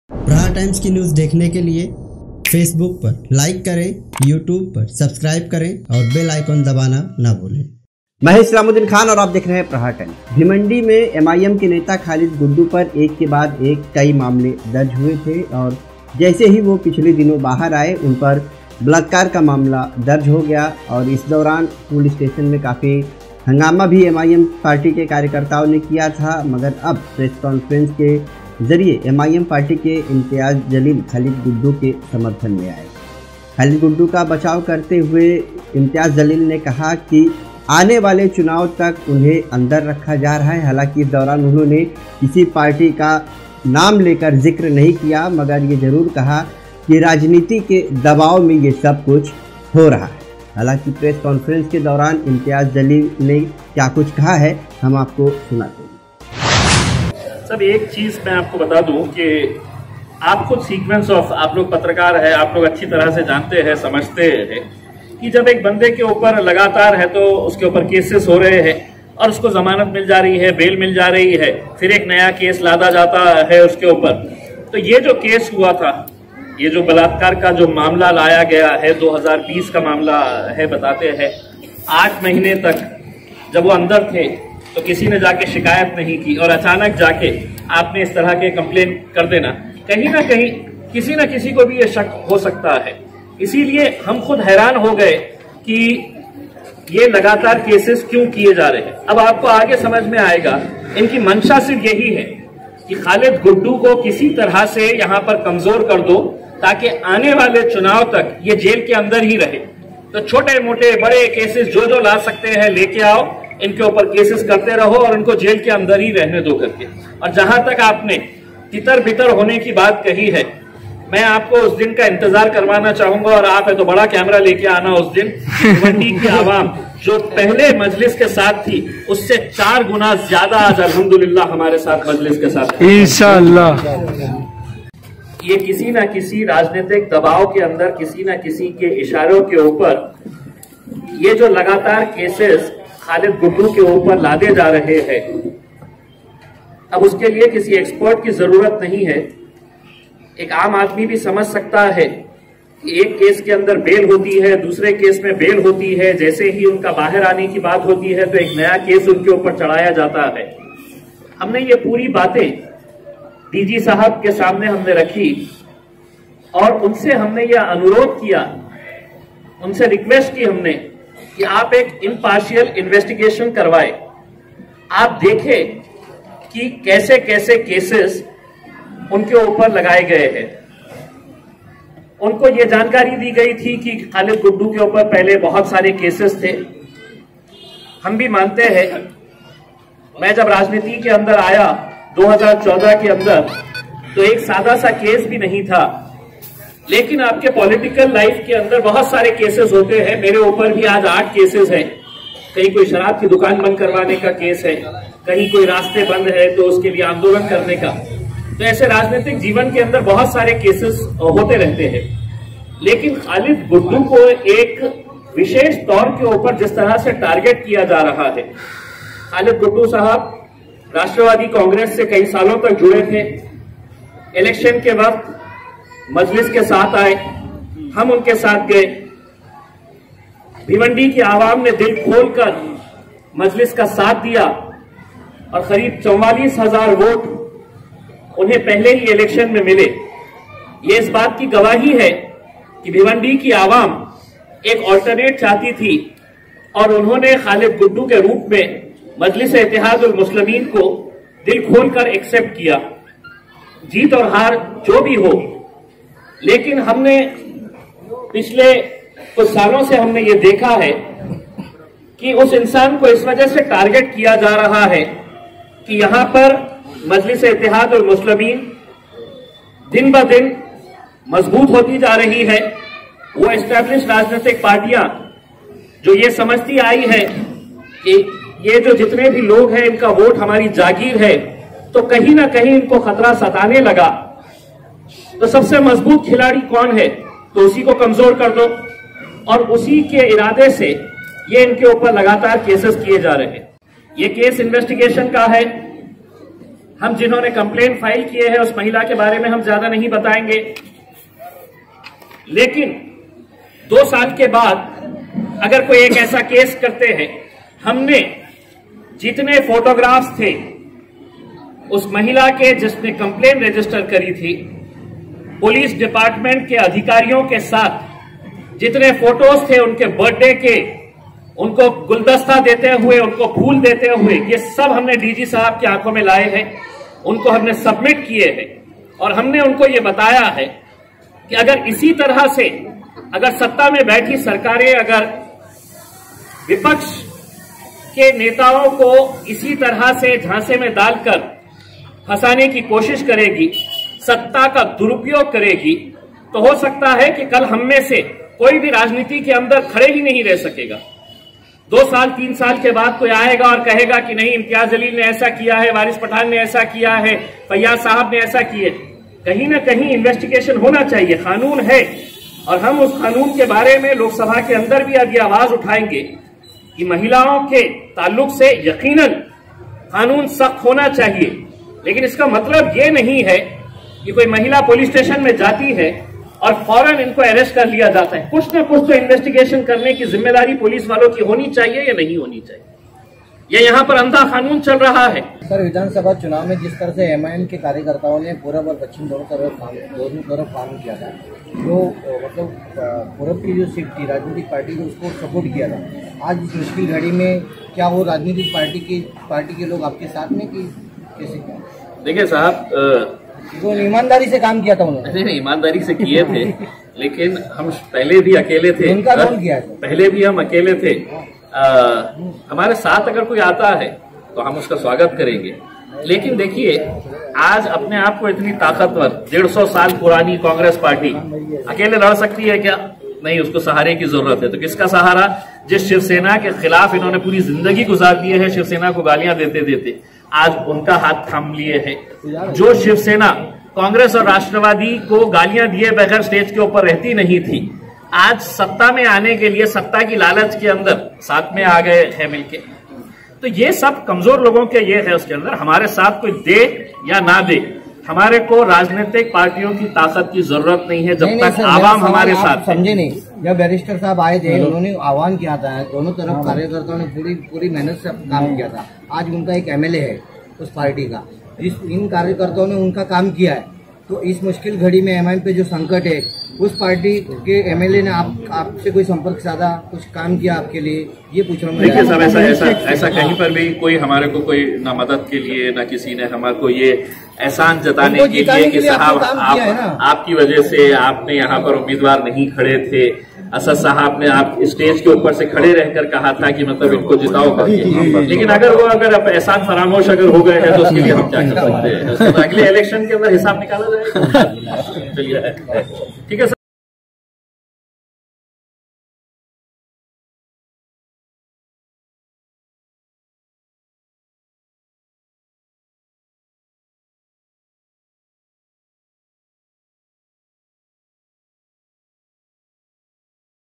ख़ालिद गुड्डू को जेल में रखने का राजनीतिक दबाव,IMTIAZ JALEEL|| Islamuddin Khan || Aapka Prahar Times
ख़ालिद गुड्डू को जेल में रखने का राजनीतिक दबाव,IMTIAZ JALEEL|| Islamuddin Khan

भिवंडी ख़ालिद गुड्डू के समर्थन में इम्तियाज ज़लील ने बिना किसी पार्टी का नाम लिए कहा चुनाव तक जेल में रखने का दबाव है